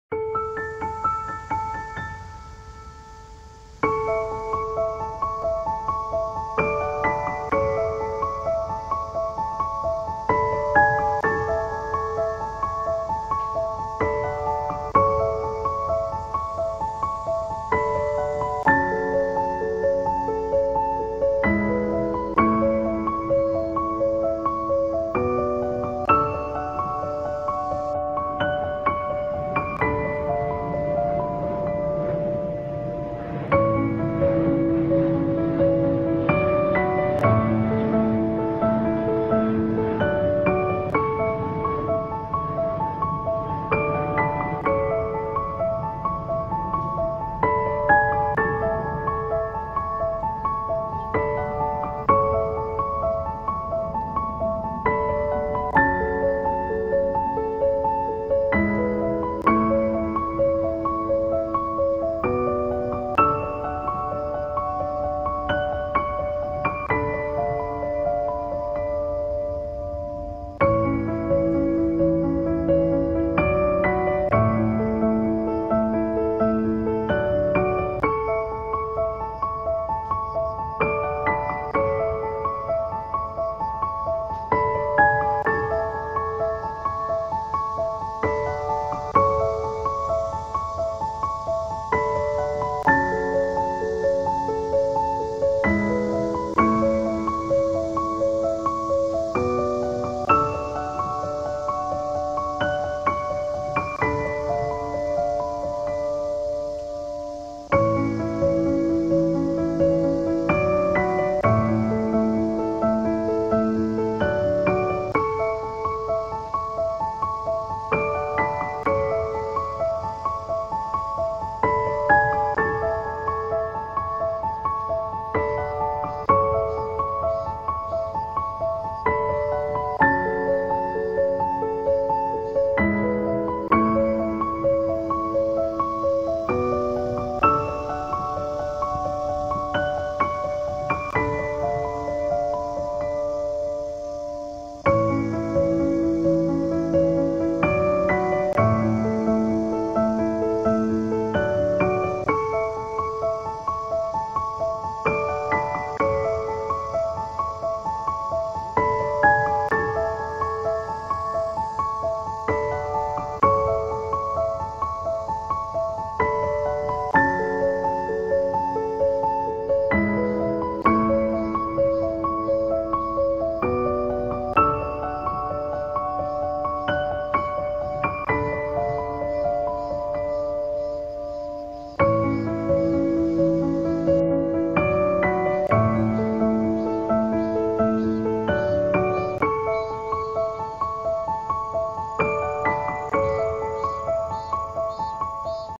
백일홍꽃밭에 산제비나비
산제비나비
나비목
호랑나비과.
학명Papilio maackii계동물문절지동물
강: 곤충류
목: 나비목
과: 호랑나비과
멸종위기등급미평가(NE : Not Evaluated,
 출처 : IUCN)크기날개길이 41∼75㎜서식장소계곡, 평지분포지역한국, 일본, 사할린, 아무르, 중국, 타이완
네이버 검색

 감사합니다